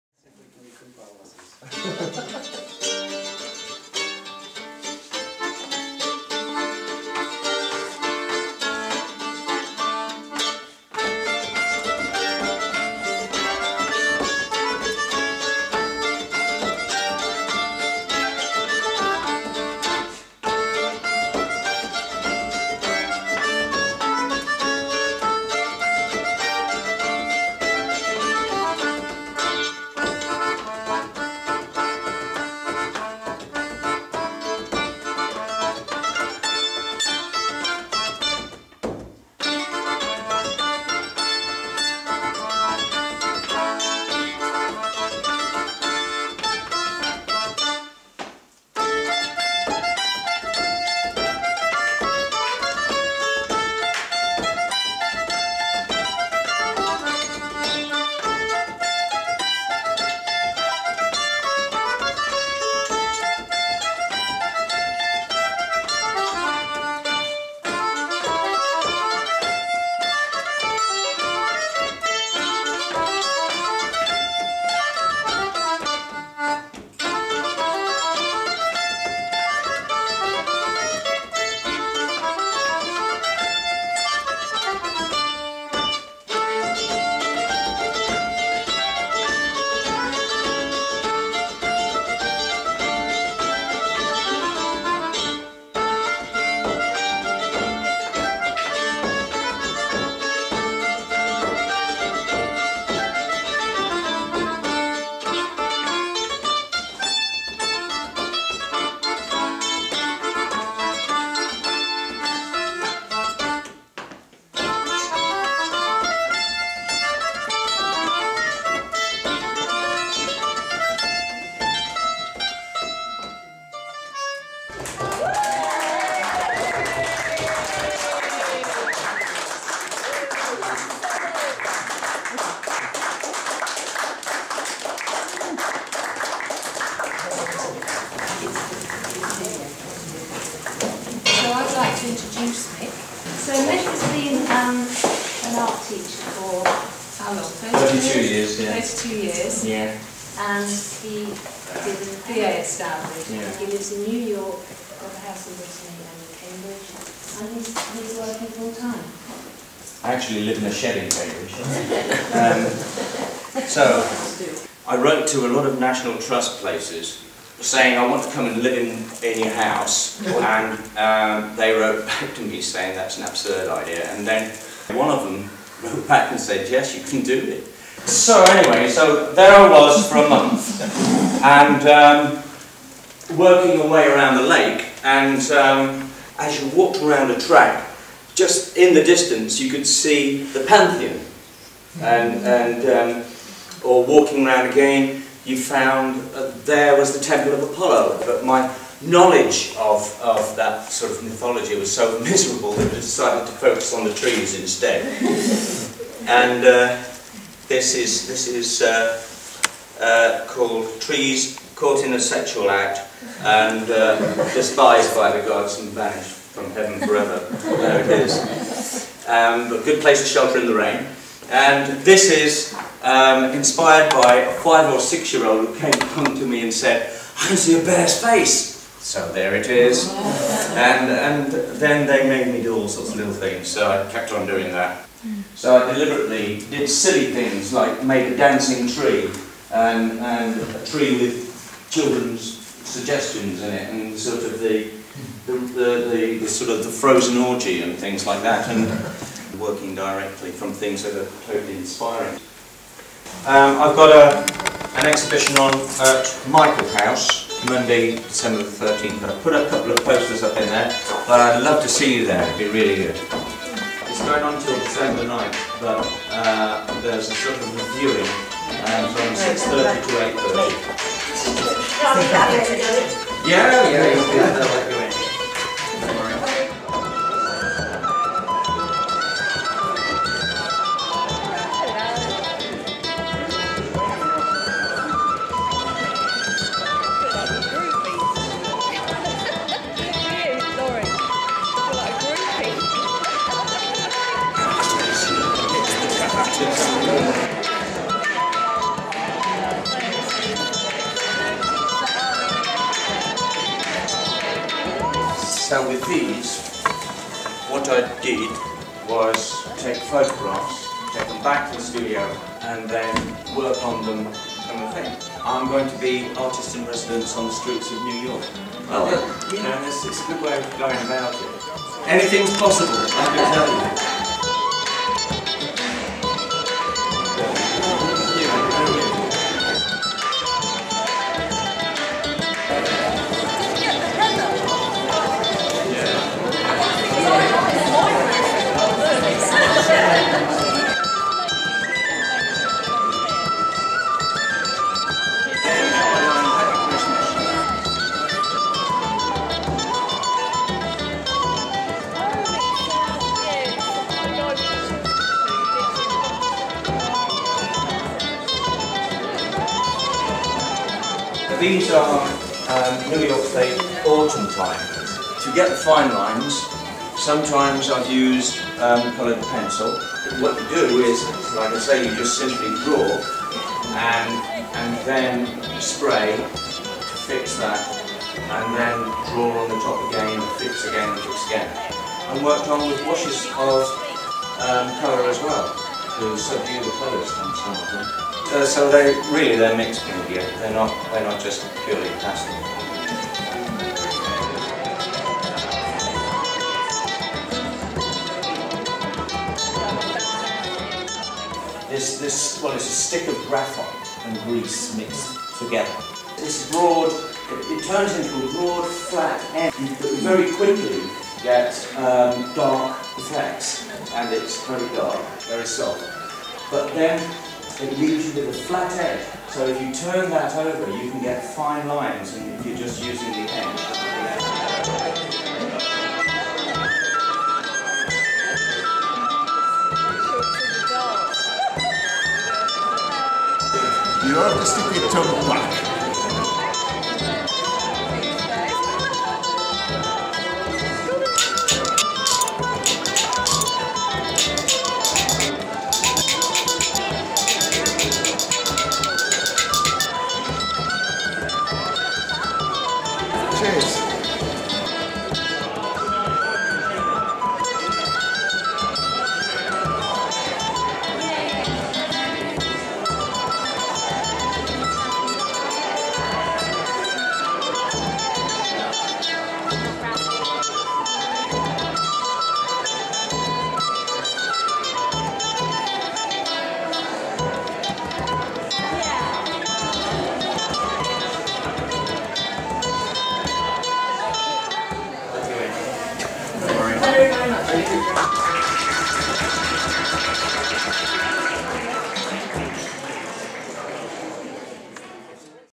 Mick Abbott - Paradise And Other Places
Mick gives a talk at Cambridge Bhuddist Centre and launches his exhibition "Paradise And Other Places" at Michaelhouse Church in Cambridge.